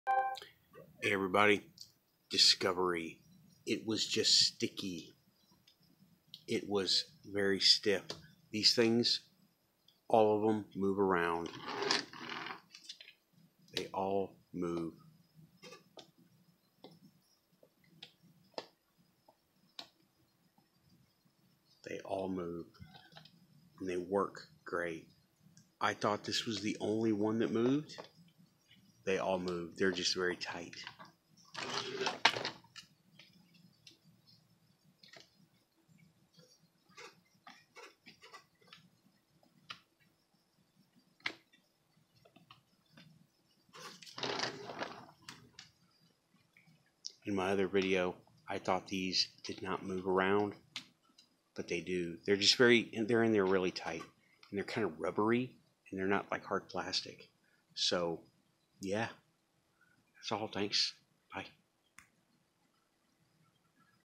Holo projectors do move :)
The holoeyes do move all around as ball joints should, just figured this out right now